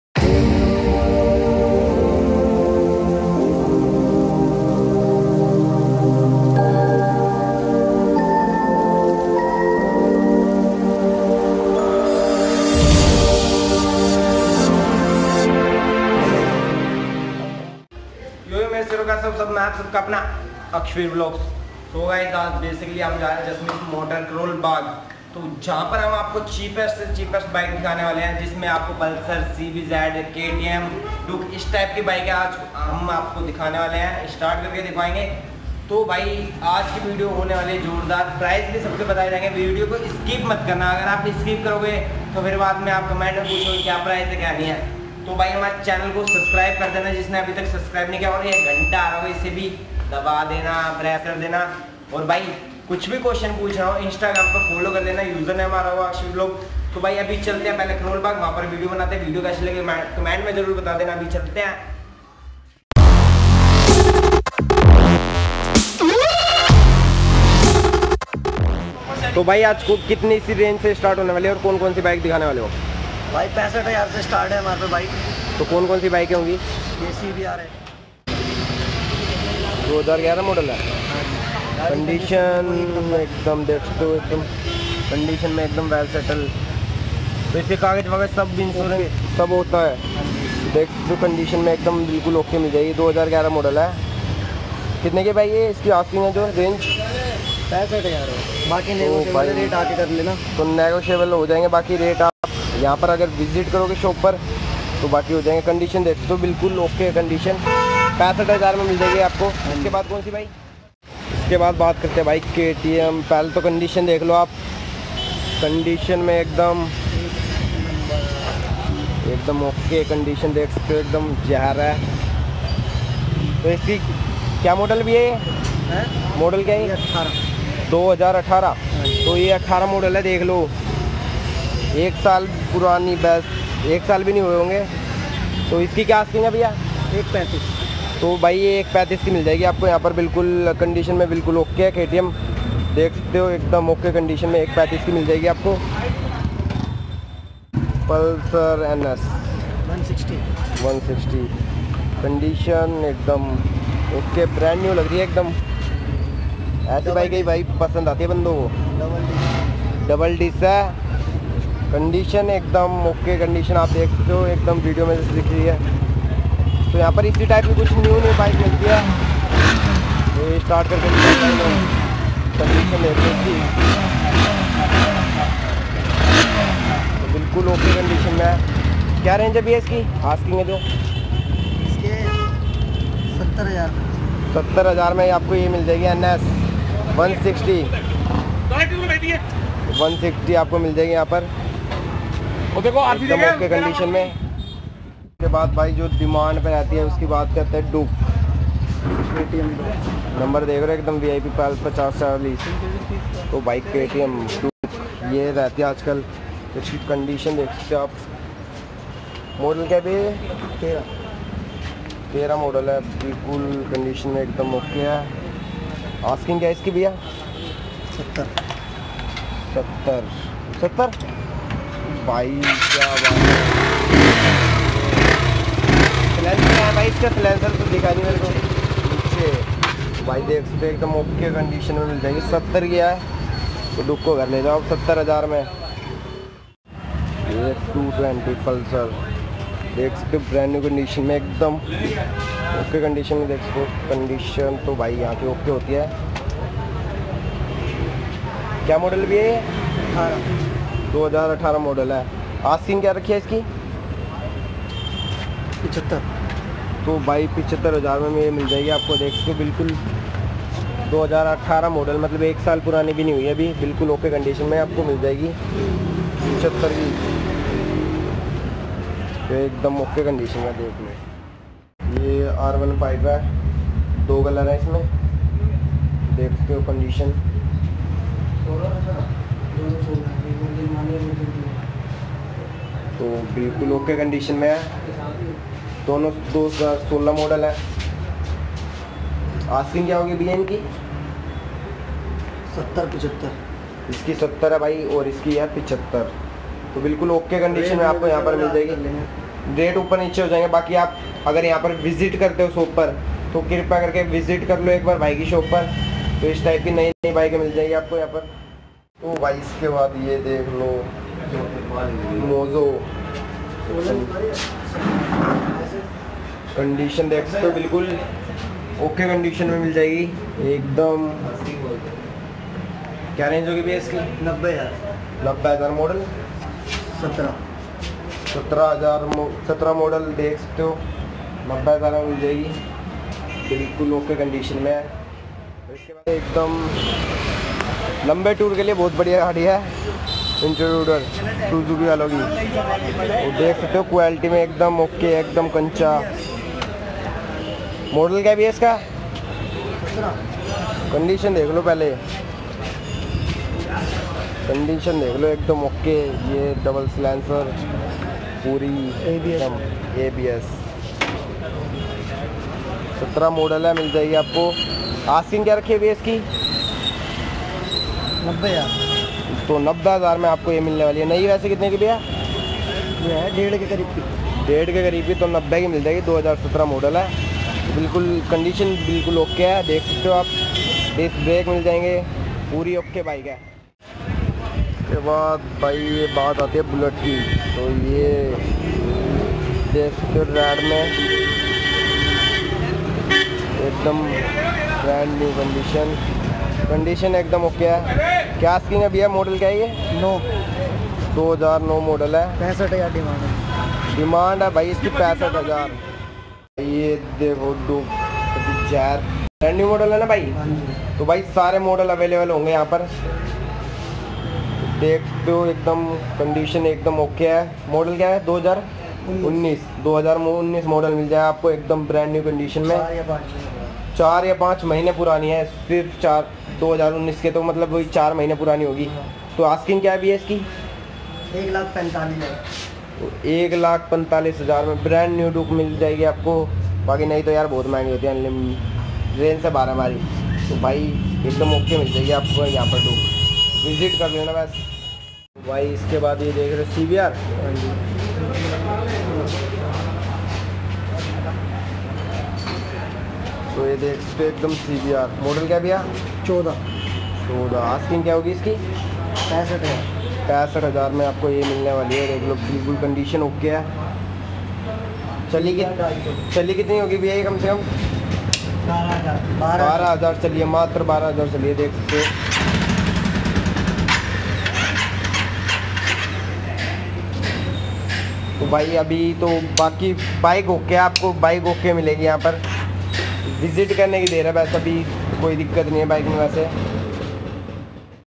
All Second Hand Bikes in ₹20,000 Only | Bike Market Delhi | KTM DUKE Pulsar with Akshveer Vlogs
SHOP Name - Jasmeet Singh

Shop owner -  Jasmeet Moters

Address : 1625/33  Karol bagh New Delhi - 110005

Akshveer
Akshveervlogs